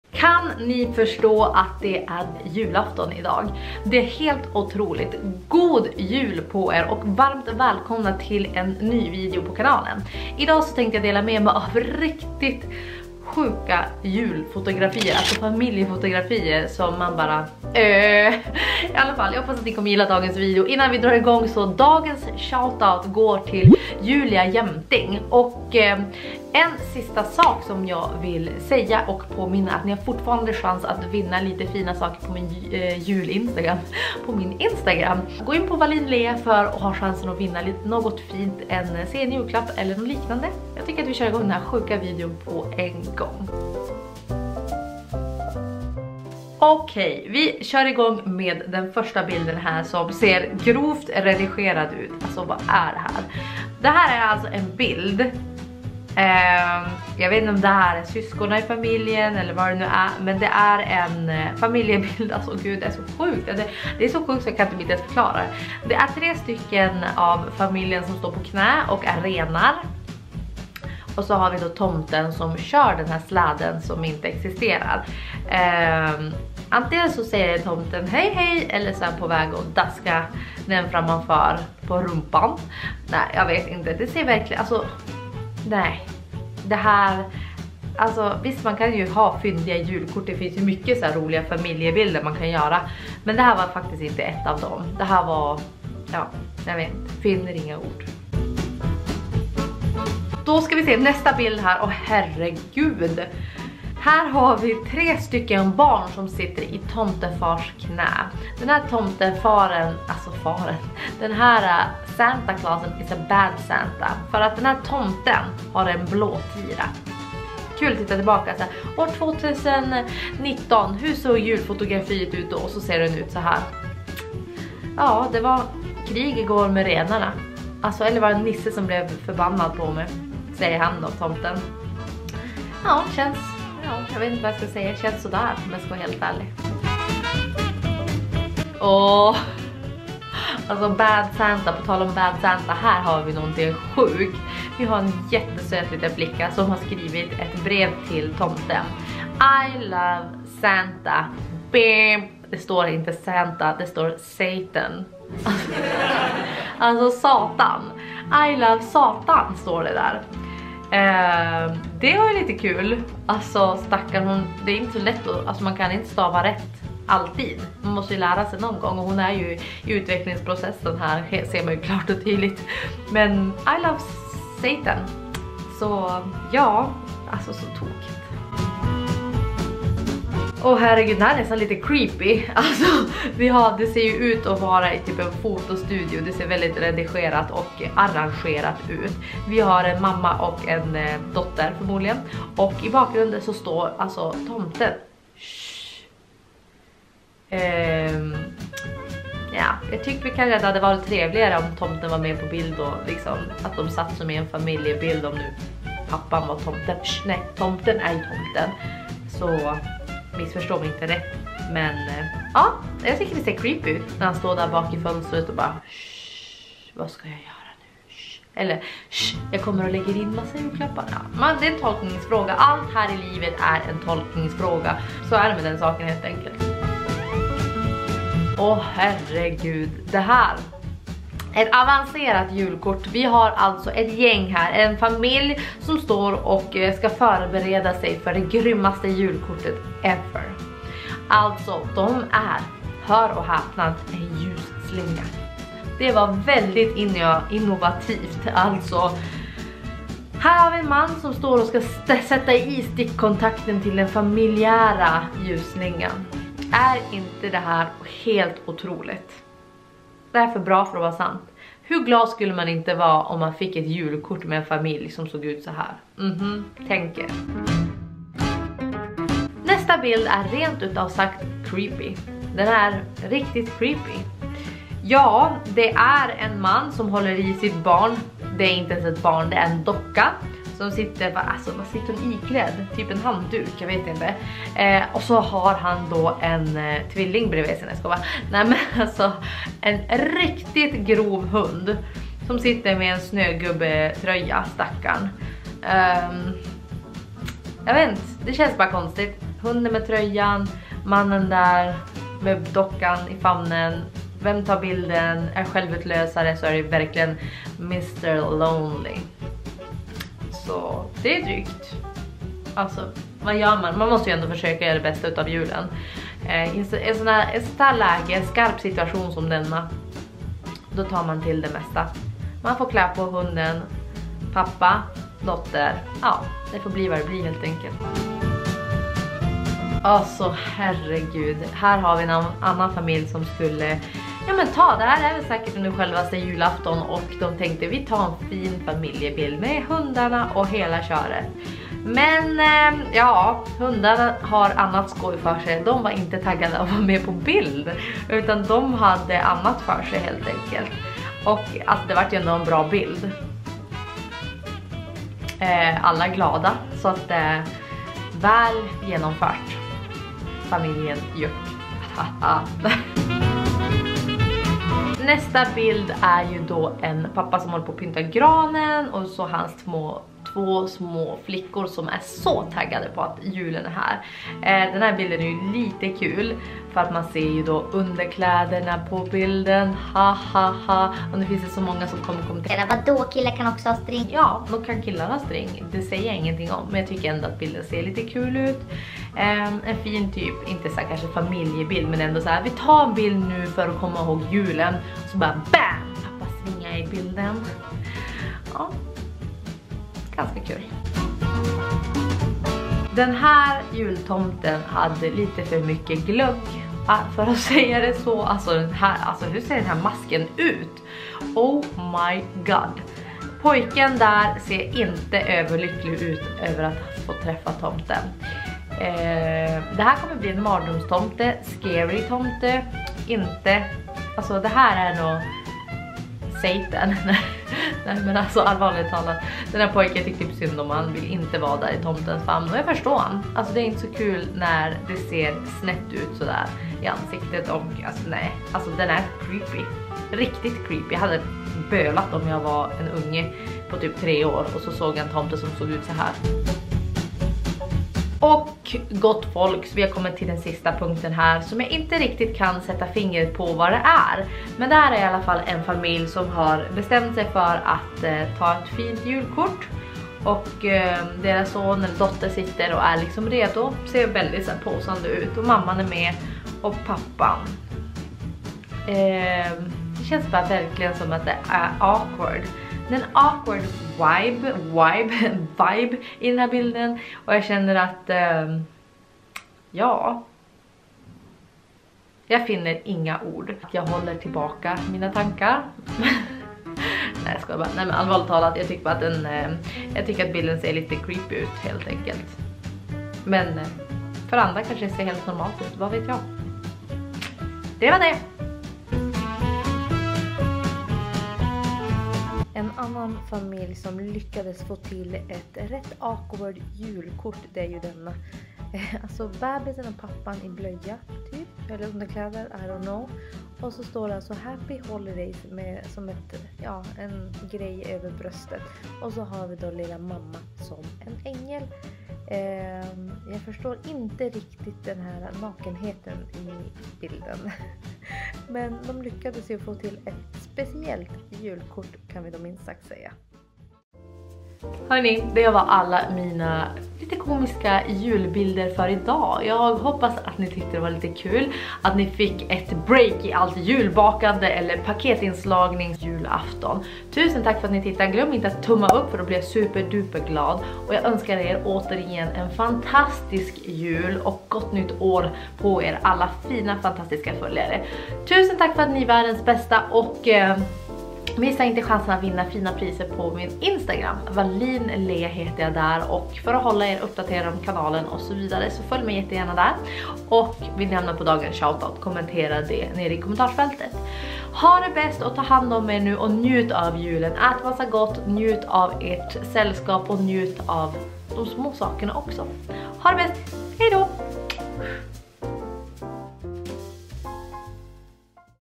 10 STELA bilder från julafton..... (GOD JUL!!)
10 STELA bilder från julafton. (GOD JUL!!) Ja allt från underklädsvisande pappor till blåslagna tomtar.. De finns alla typer av julfoton.

Hej och varmt välkomna till årets enda julvideo. Hoppas ni är taggade, fattar ni att det är JULAFTON 2020 året som har varit det längsta året i historien,  (det känns ju så iallafall) Hoppas ni kommer att gilla denna video, jag önskar er en riktigt god jul. Ta hand om er! För vloggar kolla in min engelska kanal PUSS